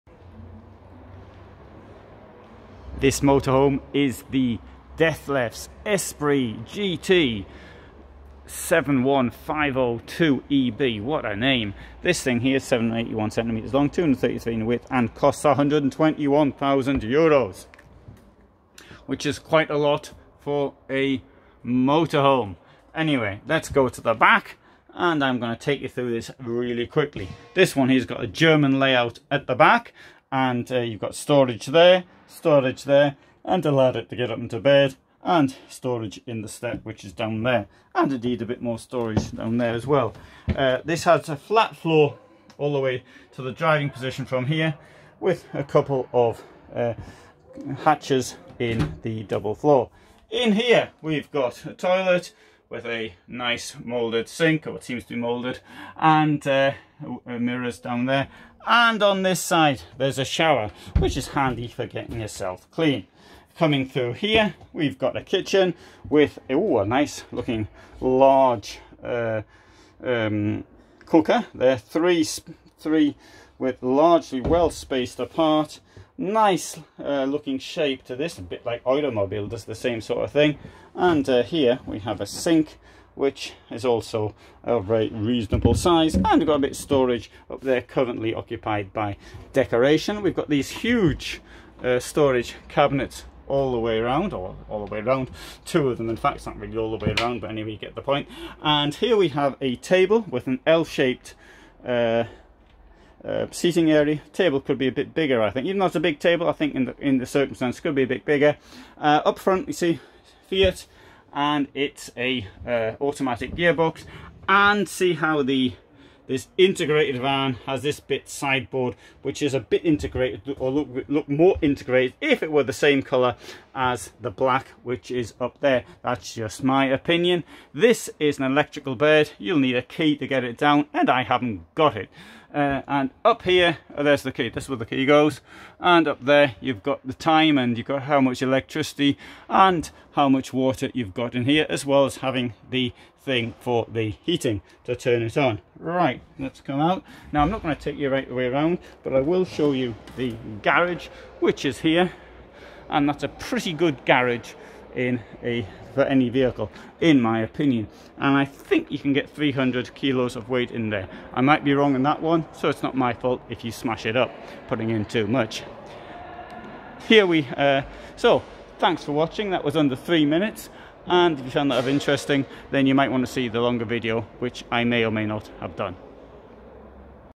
120k Dethleffs motorhome. Short tour of the Esprit GT I 7150-2EB
This Dethleffs motorhome is the Esprit GT I 7150-2EB - what a name!  This is an integrated motorhome which comes at quite a price.  Positive features include the double floor with storage built in and an enormous garage.

The Esprit has always combined modern design with comfort. For 2022 it has been completely redesigned. Thanks to its new, more than 26 cm high, loadable double floor, it not only offers plenty of storage space , but has even refined its basic recipe as a winter-proof all-year-round companion .

The Esprit adopts the graceful, clear and flowing lines of Dethleffs' own design language, which is primarily reflected in the aerodynamic front and the slightly retracted rear of the integrated models.

The interior of the revised Esprit presents itself openly and with a fresh, Nordic breeze, which exudes a natural component by means of profiled wood decor and thus radiates a homely comfort despite the modern design.

Quick overview:
Interior in Scandinavian look.
Winter resistance thanks to raised floor technology and frost-proof installation
Fully equipped kitchen with cooker, drawers with soft-close pull-in and large fridge-freezer combination (can be opened on both sides if desired)
Electrically adjustable fold-down bed with 200 x 150 cm surface, mattress and slatted frame
Wide-gauge low-frame chassis with 145 mm lowering and a permissible gross weight of up to 4.5 t